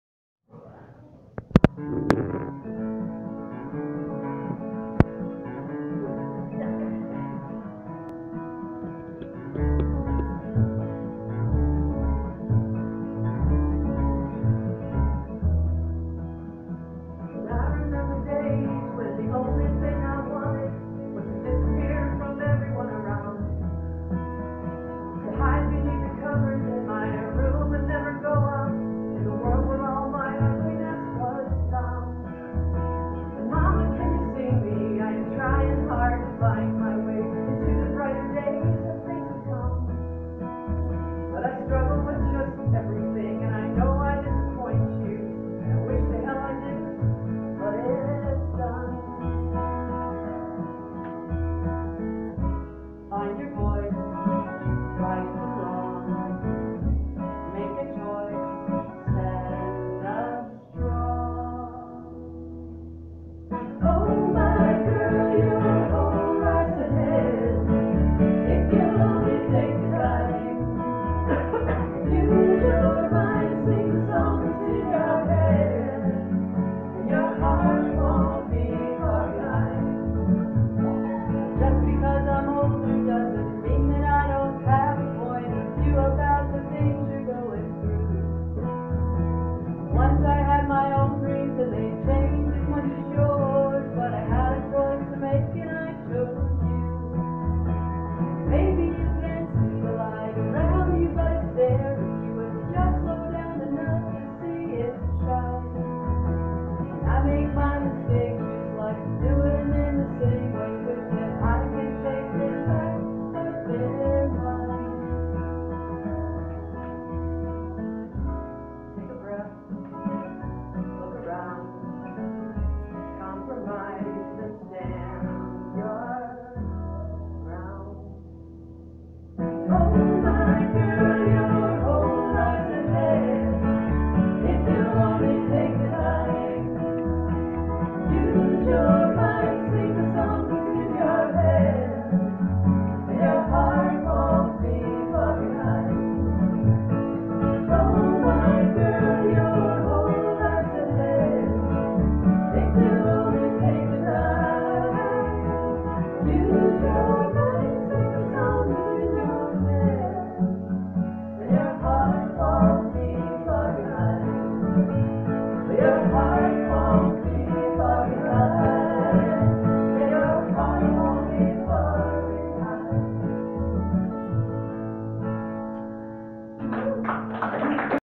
Judy Painter & Rose Kimball preform a song about daughters at a Dixon, Illinois house concert.
Judy Painter & Rose Kimball preform a song about daughters at a Dixon, Illinois house concert on 07/22/2016.